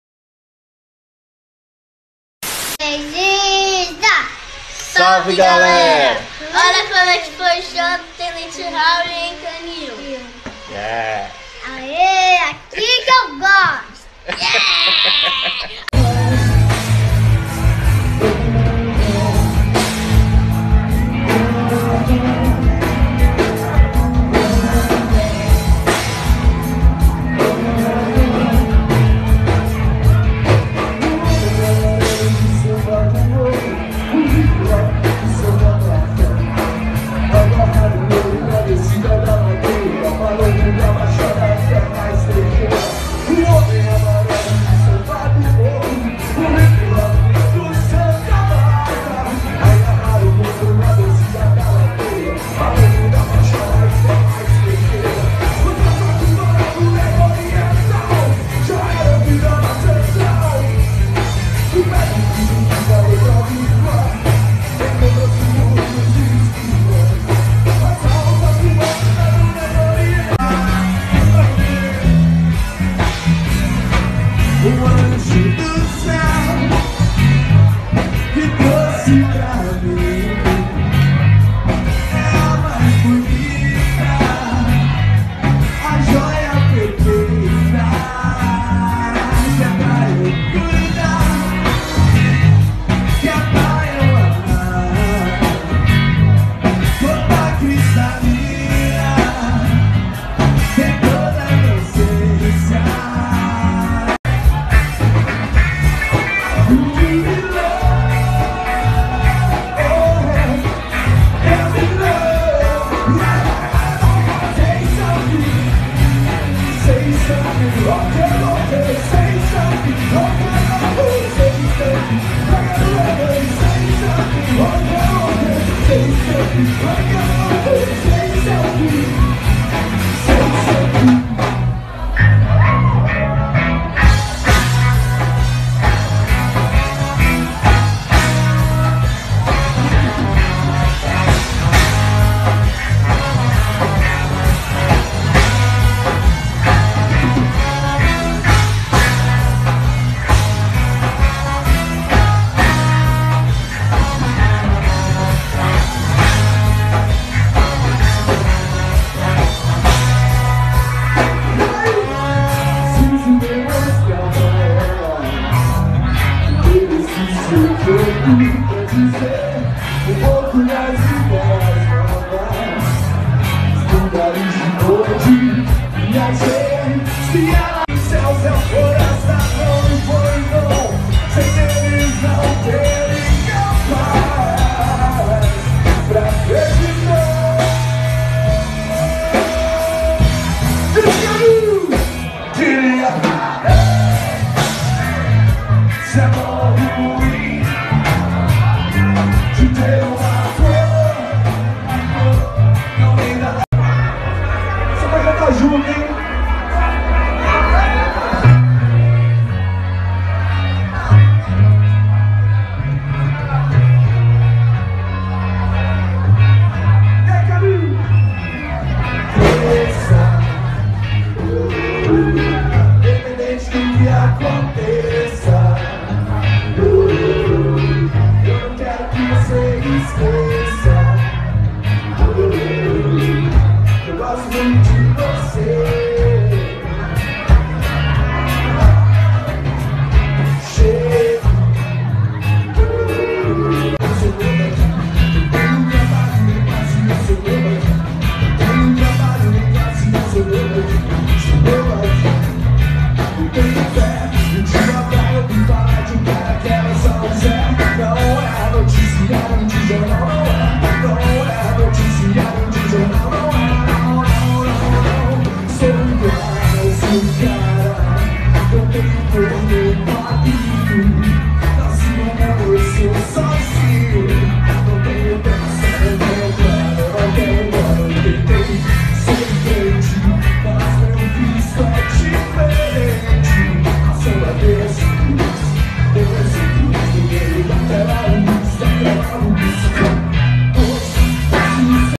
TENENTE HAOLE - CANNIL CLUB - PERUIBE / ABRIL 2017
casa lotada, vibe positiva, TENENTE HAOLE NA CASA!!!